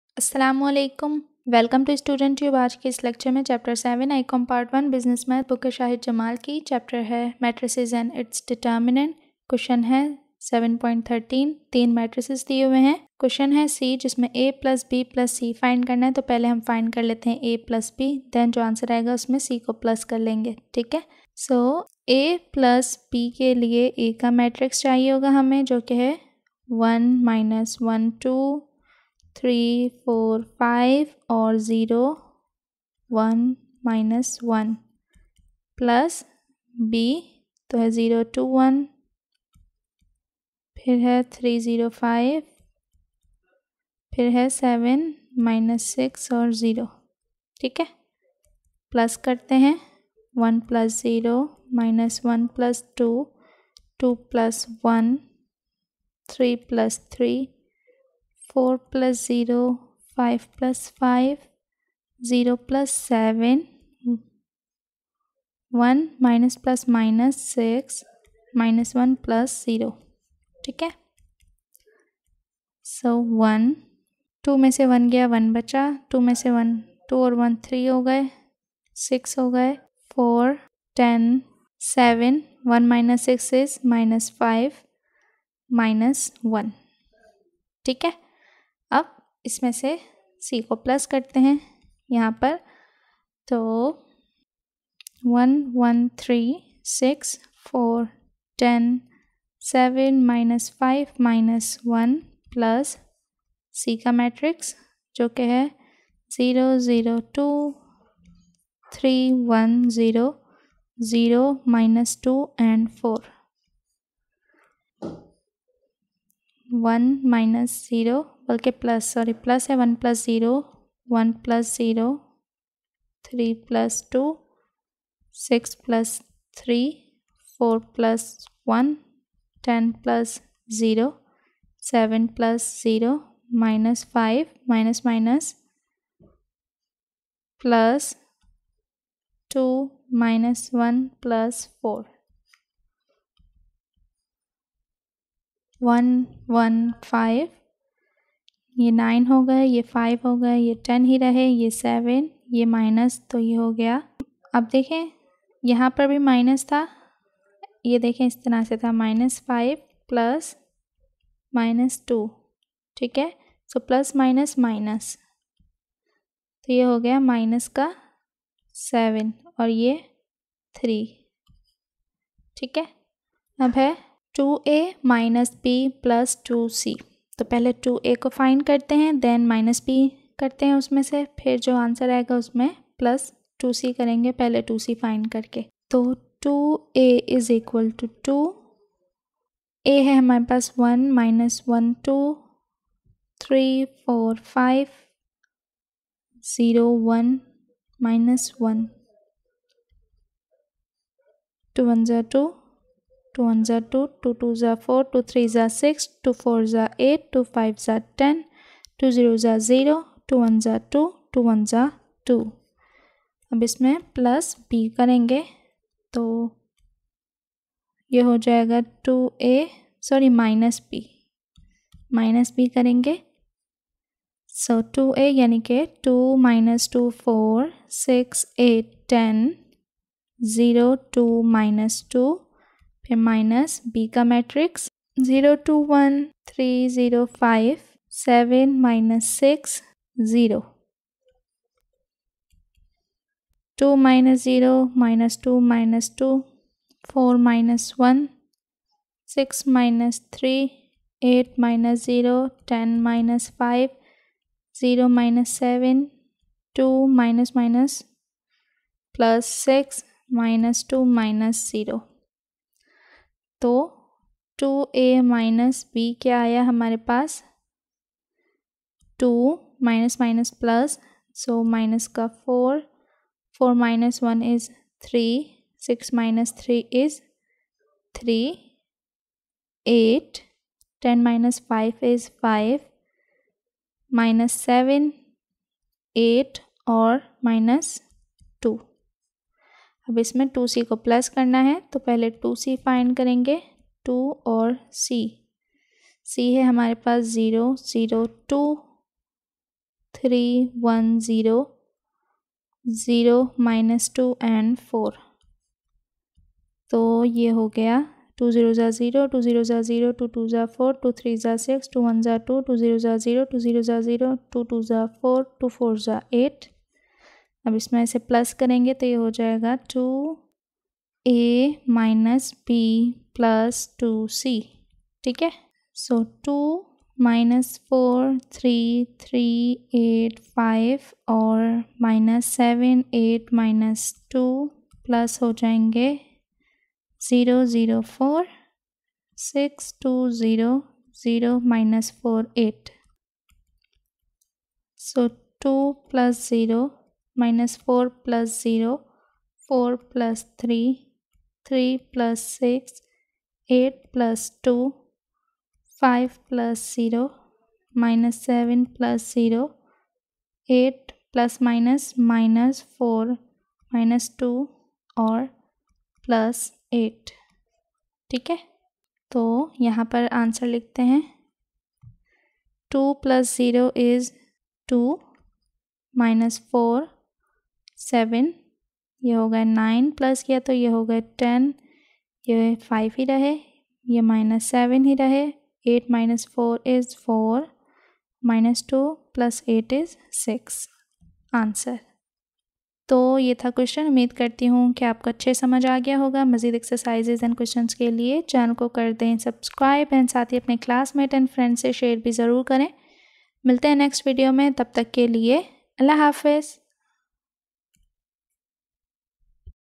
Chapter 7 - Exercise 7 - Q 7.13 part c & d Solution (I.COM Part 1 Business Math - Sindh Board)
Today's student tube lecture is about chapter 7 - Exercise 7 - Q 7.13 part c & d (I.COM Part 1 Business Math - Sindh Board Education)

This chapter is about the Matrices and Determinants. In this chapter, we will learn how to add, subtract and multiply matrices. How to solve an inverse matrix, Cramer's rule, and many more. This lecture is for all 1st-year commerce students. I will suggest you buy Shahid Jamal (Business math book for I.COM part I) and you are good to go.

and

detail.